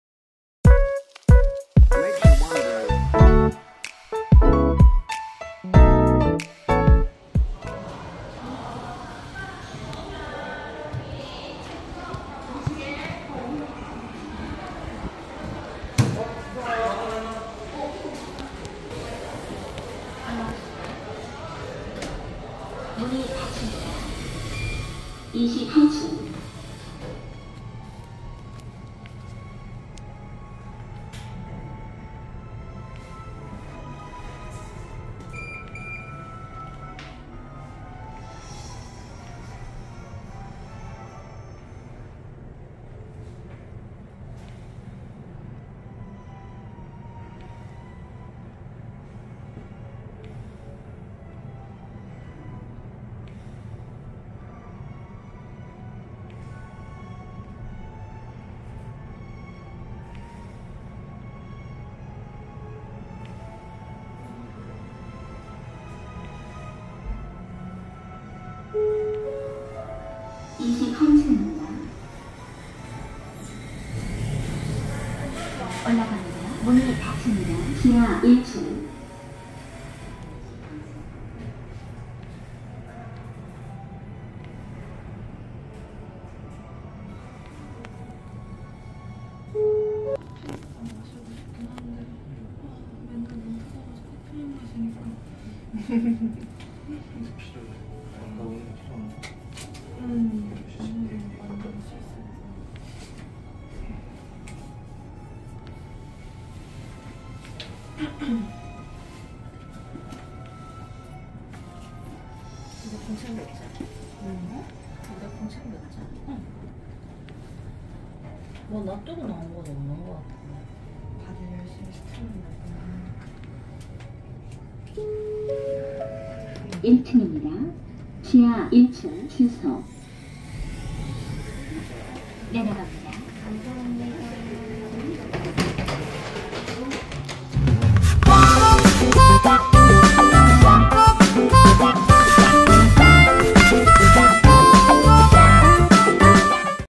부산광역시 영도구 봉래동1가 영도라발스호텔 OTIS엘리베이터 탑사기
2022년 5월 29일 영상분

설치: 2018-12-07
모델: GM2(TD)
종류: 장애인용
속도: 150m/min

이날 주말이라 이용객이 폭주했습니다. ㅡㅡ

덕분에 모자이크 편집이 많은점 양해 바랍니다.

뭐 딱히 설명할거는 없습니다.;;;